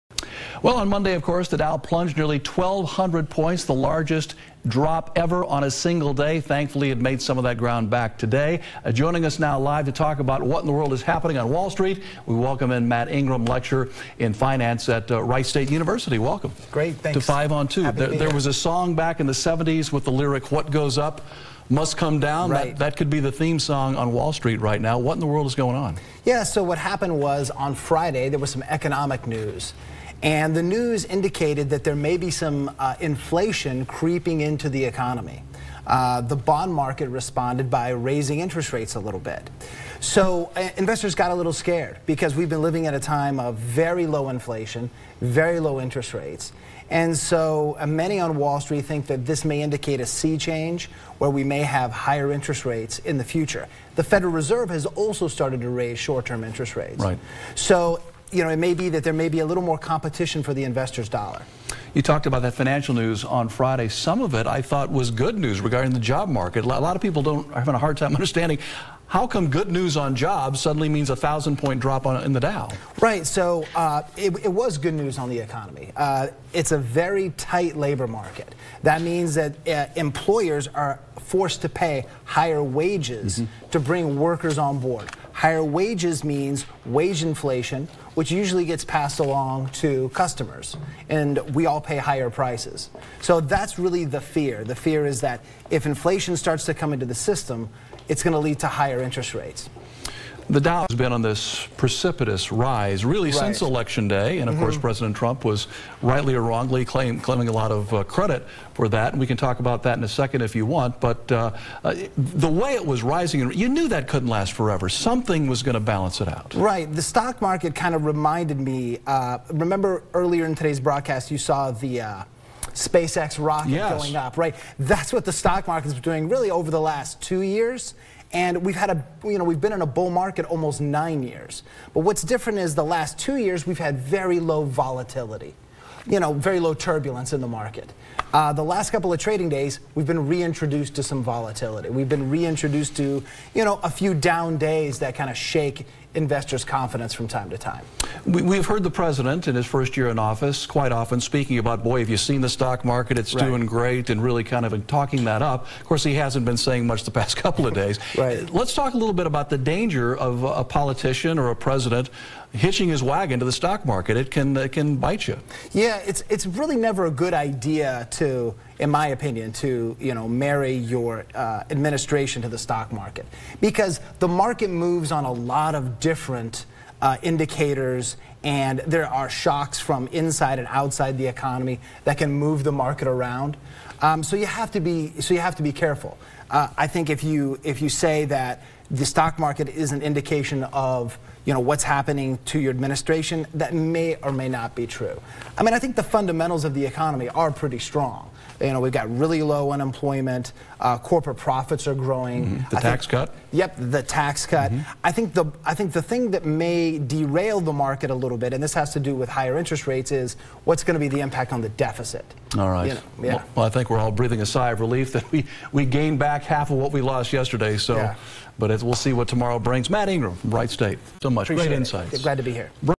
WDTN:Wright State lecturer talks about the ups and downs of Wall Street
2/6/18 WDTN